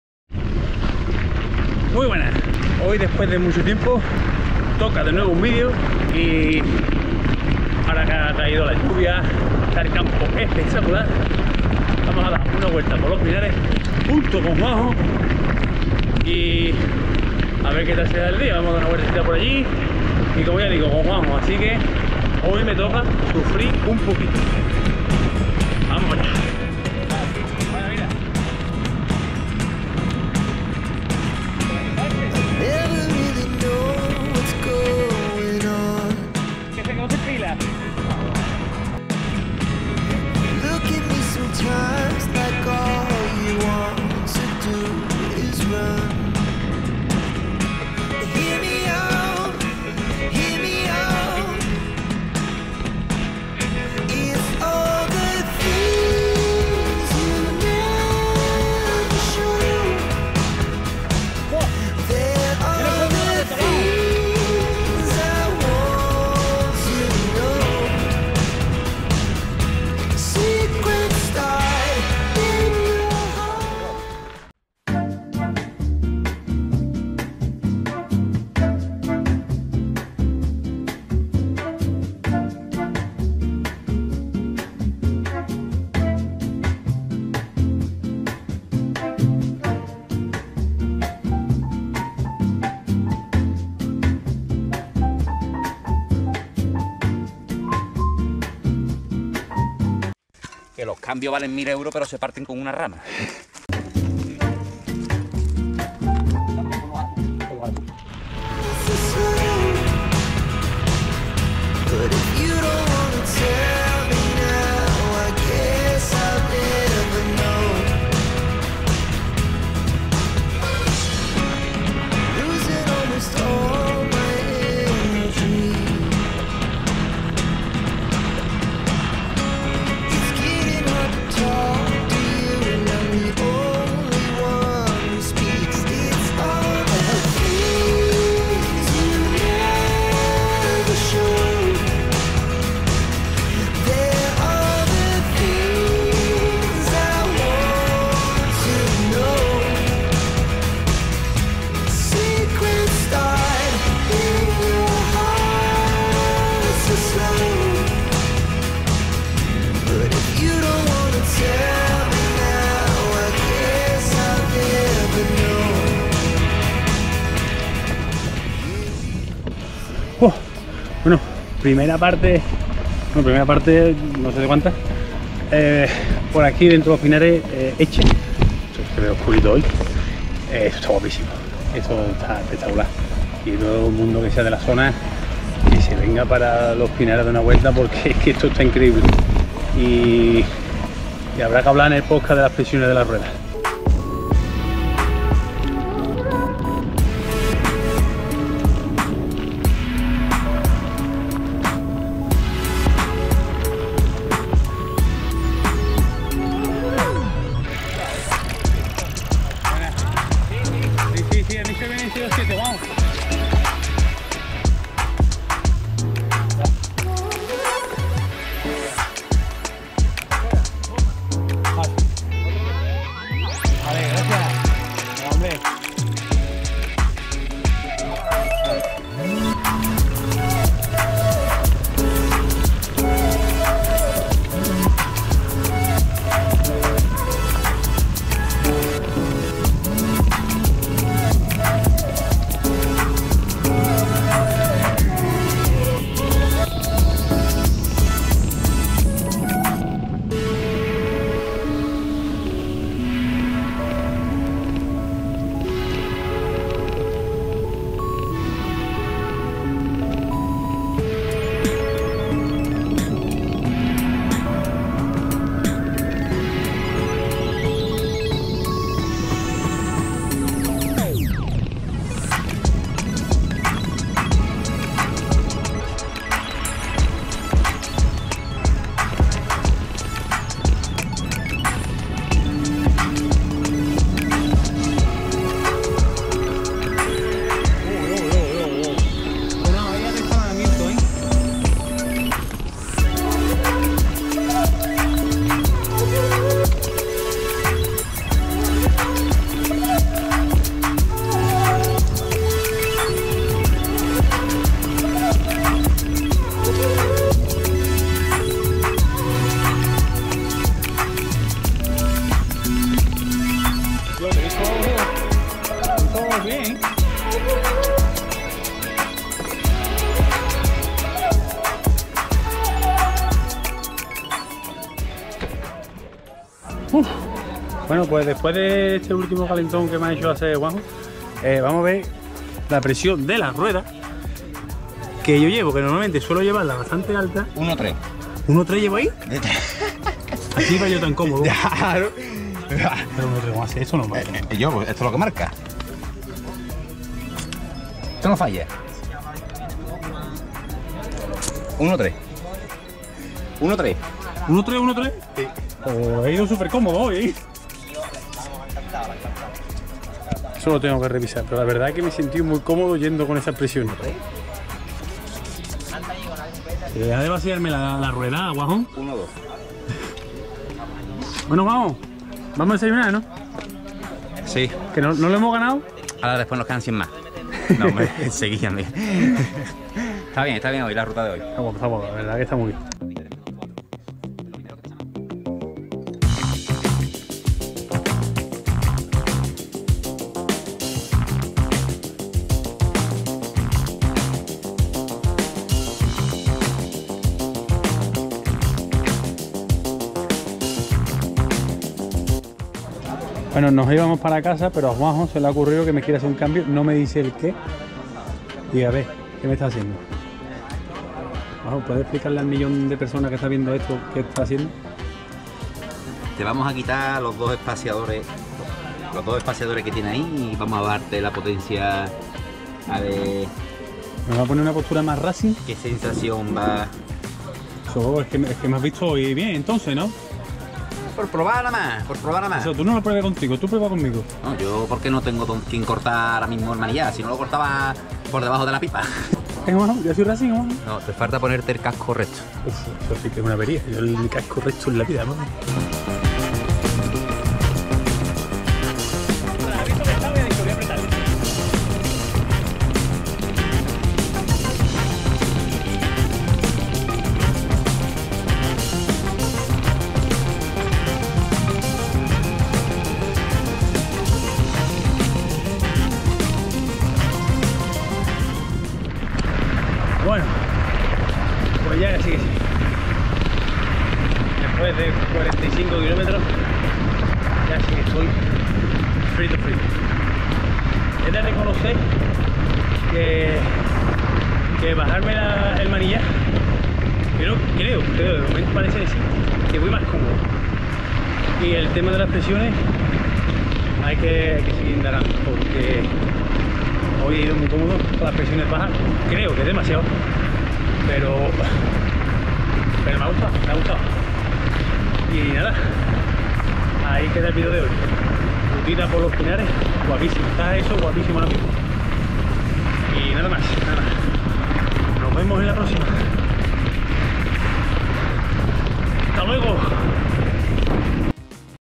DE RUTA ENTRE PINARES Y MÁS COSITAS
Después de un tiempo sin subir vídeos, me he dado una vuelta con mi amigo Juanjo por los pinares de Aznalcázar que ahora mismo es un territorio que está espectacular a da una vuelta y la verdad es que mejor no me lo he podido pasar.

También hemos estado mirando las presiones de las ruedas que es un tema que nos está dando mucho que hablar últimamente.